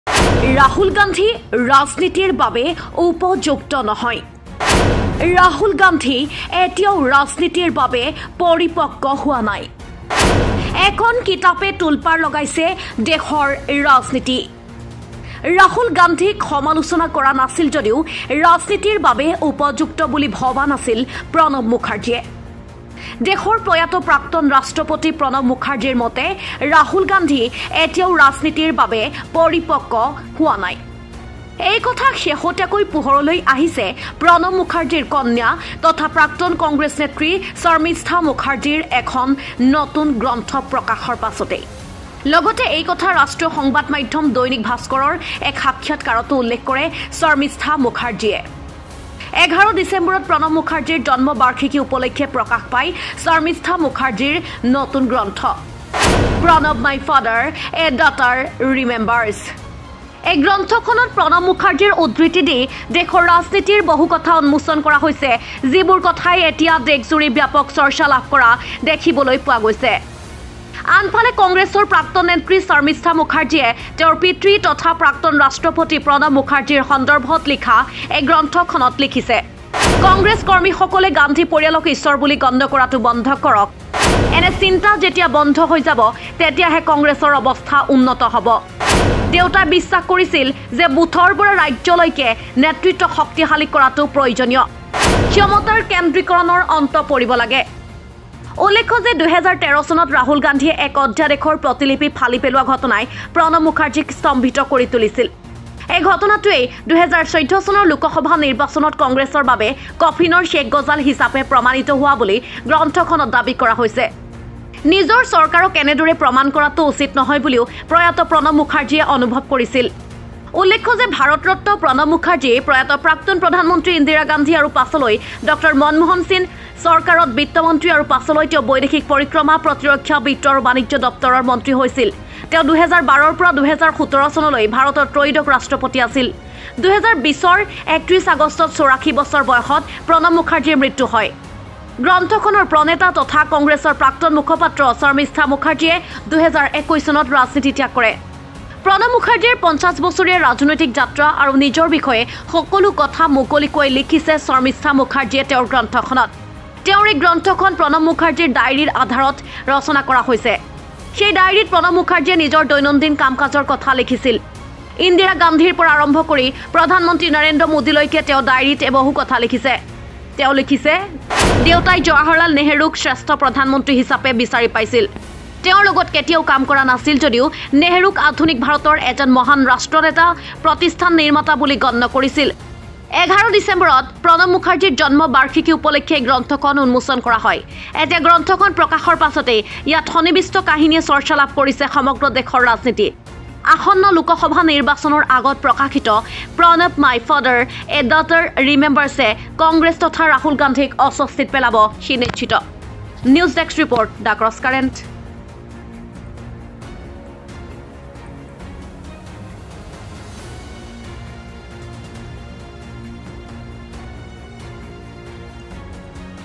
এখন কিতাপে তোলপাৰ লগাইছে দেশৰ ৰাজনীতি
"ৰাহুল গান্ধী ৰাজনীতিৰ বাবে উপযুক্ত নহয়। ৰাহুল গান্ধী এতিয়াও ৰাজনীতিৰ বাবে পৰিপক্ক হোৱা নাই।" এখন কিতাপে তোলপাৰ লগাইছে দেশৰ ৰাজনীতি।